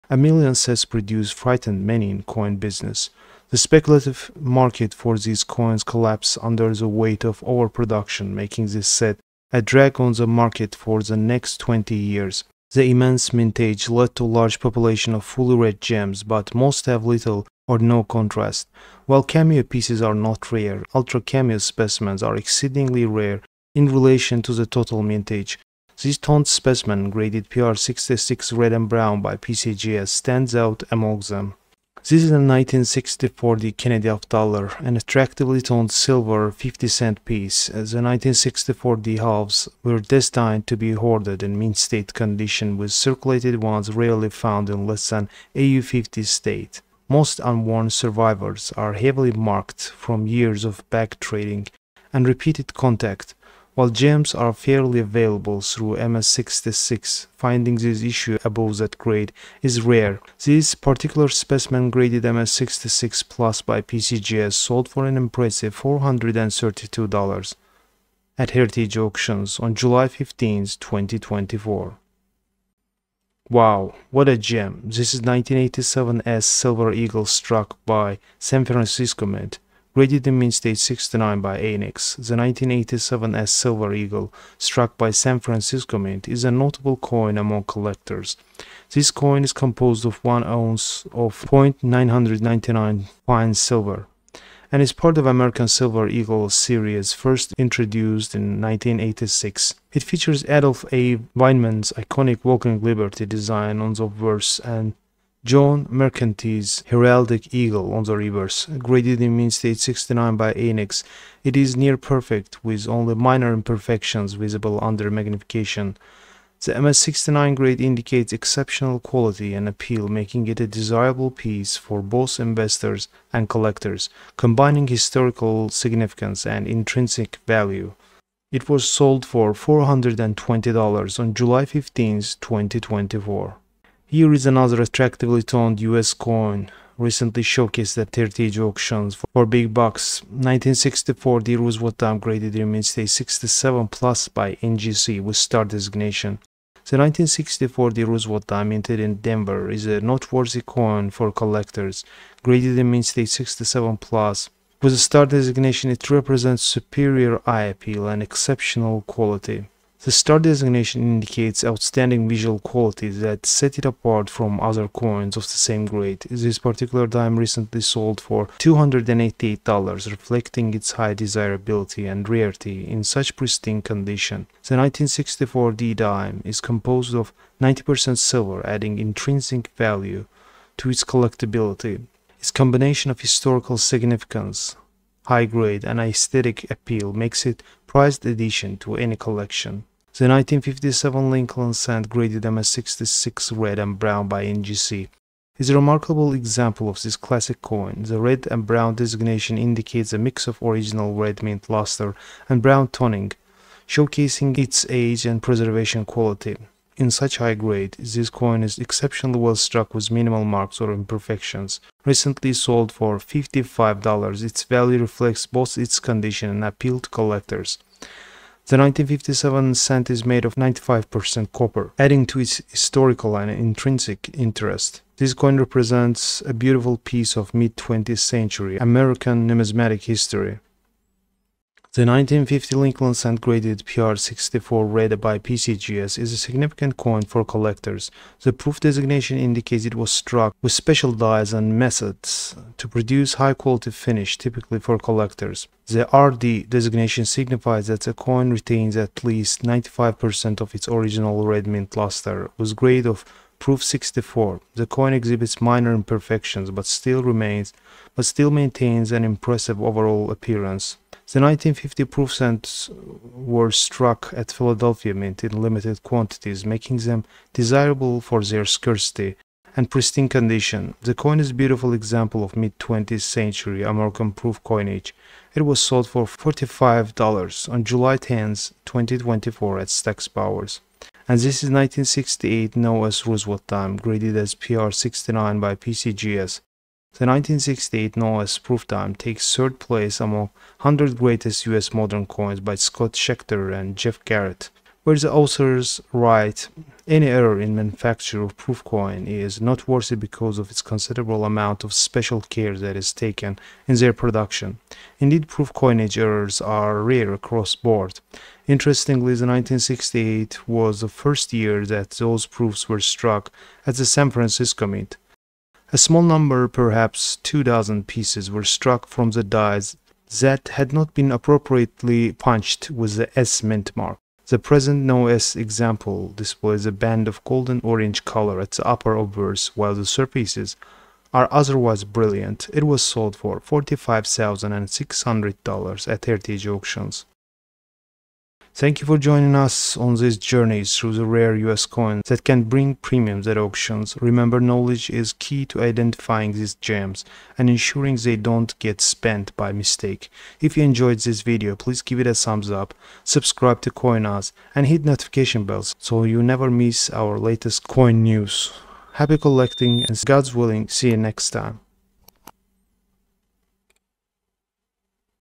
Don't Spend These Coins: Rare US Coins Worth Money!
Discover the hidden value of rare US coins in this fascinating video. You won't believe how much these coins are actually worth!